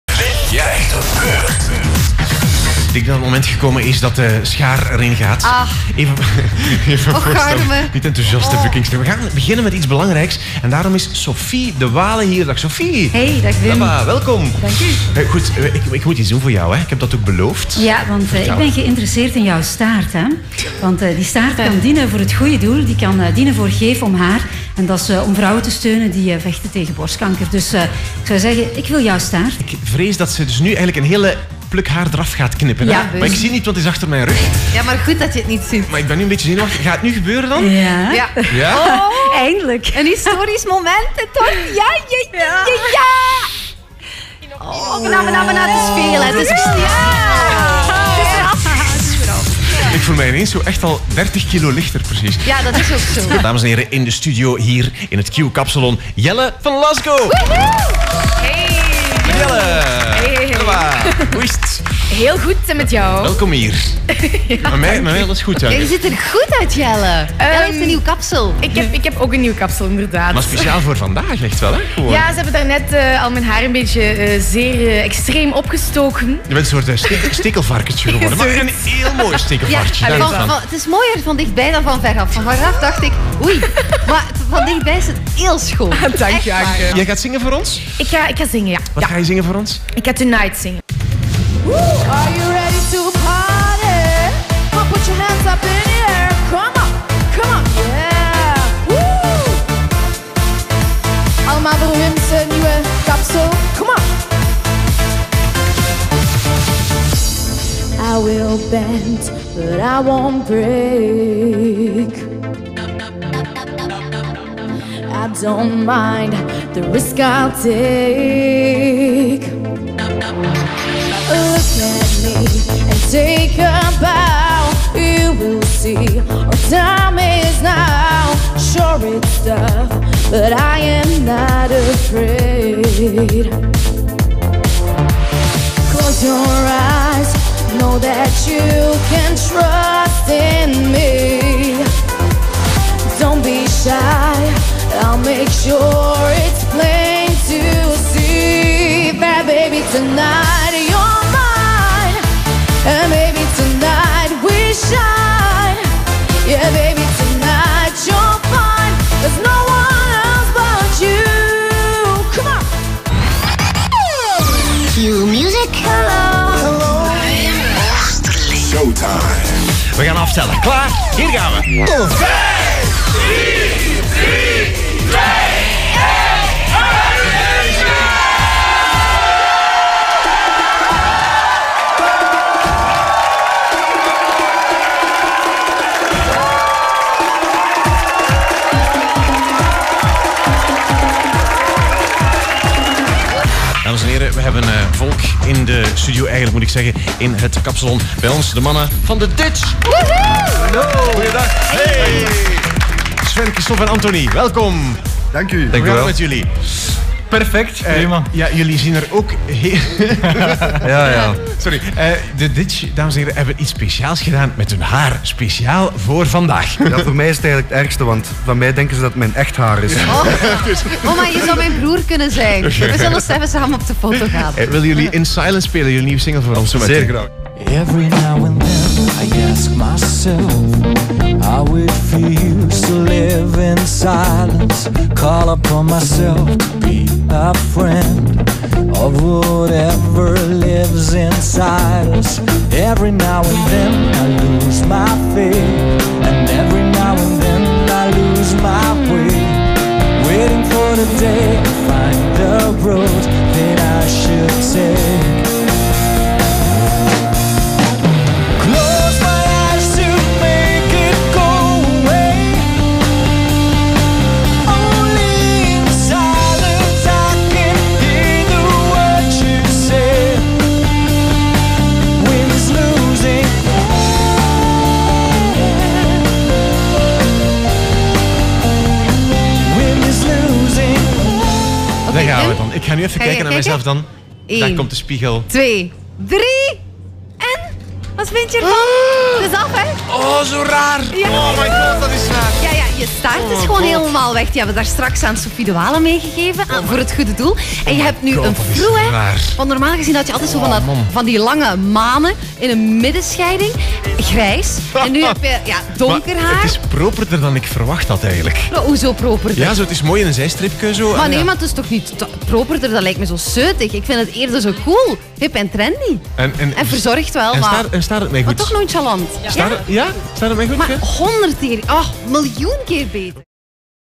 Q-kapsalon: Wim krijgt een beurt!
Wim kreeg in het Q-kapsalon jullie favoriete kapsel! In het bijzijn van Jelle van Lasgo, Sophie Dewaele, The Ditch, Jan Verheyen en Jan Schepens werd het de Ashton Kutcher-look.